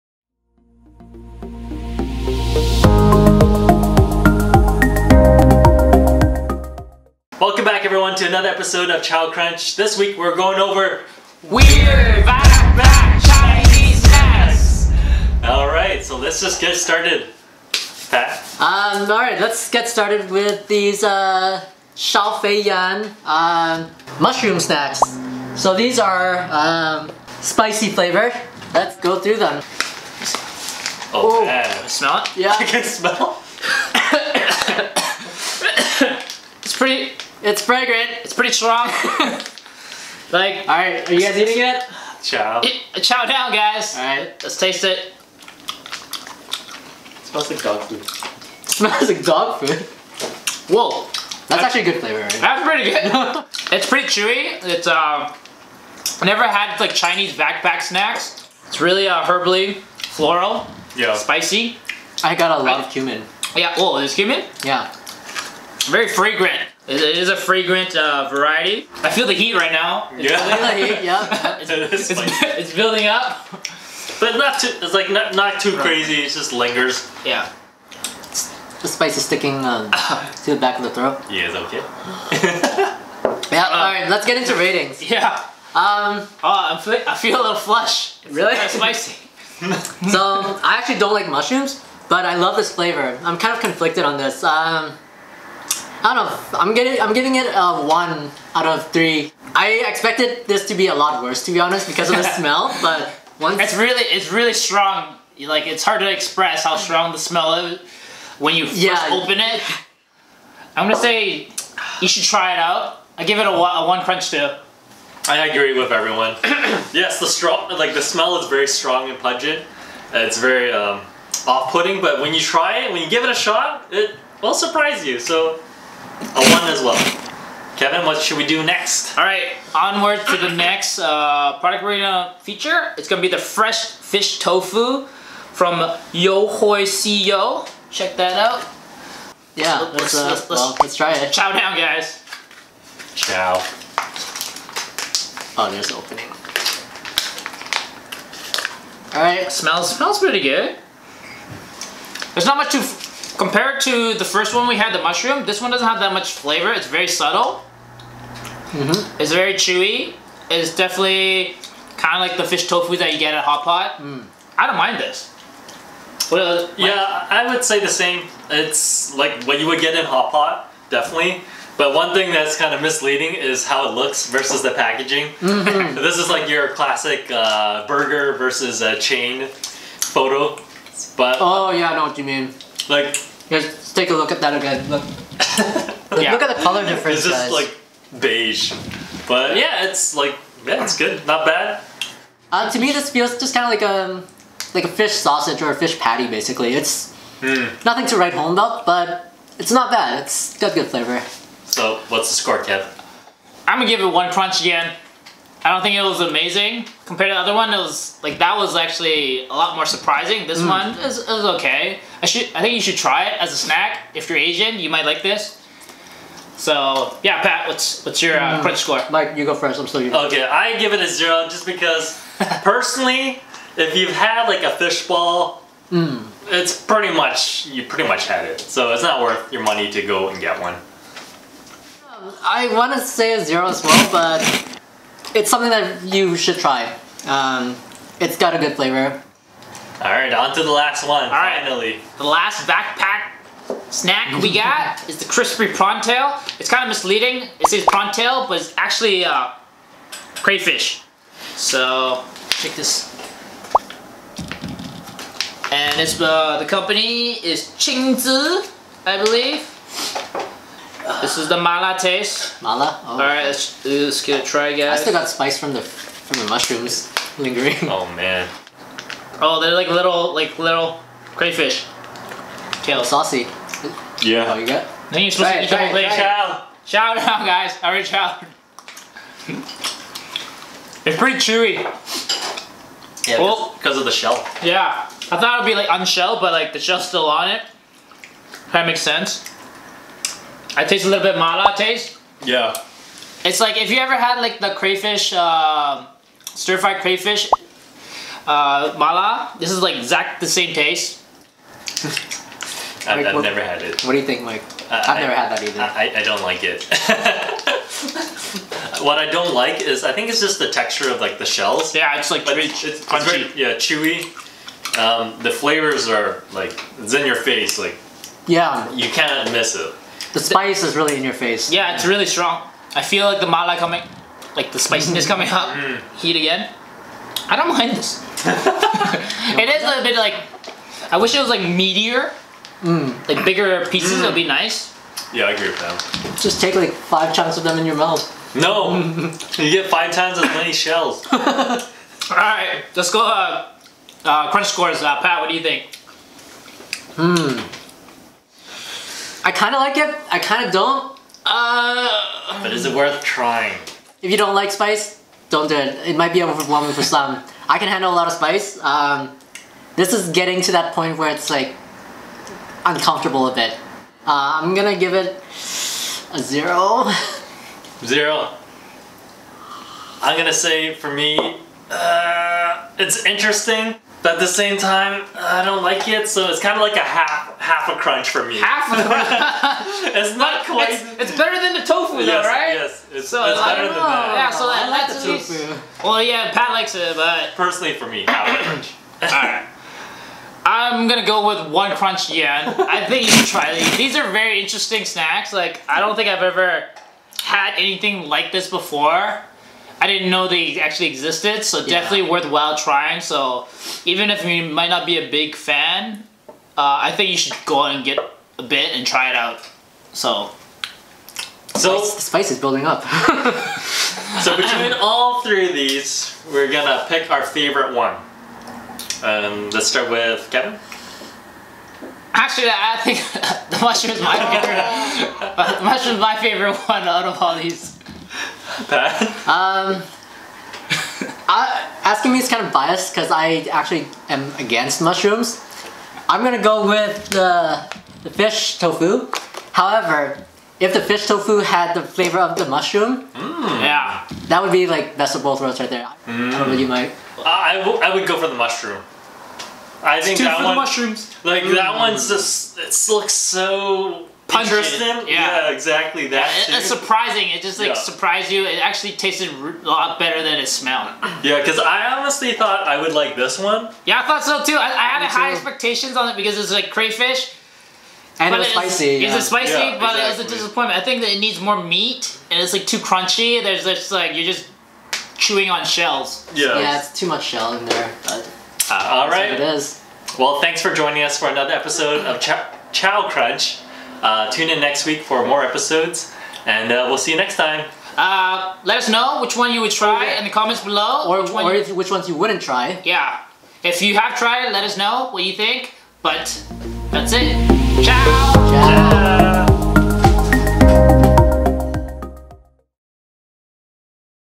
Must try WEIRD CHINESE SNACKS!!! 🦐🍢🍄
We went out and bought some vacuum-packed Chinese snacks that you wouldn't usually buy.

Timestamps:
00:00 Intro
00:23 Xiao Fei Yan Mushroom Snacks
02:13 Yohosiyo Fish Tofu
04:01 Qing Zai Mala Flavor Crayfish
06:54 Final Verdict
08:00 Closing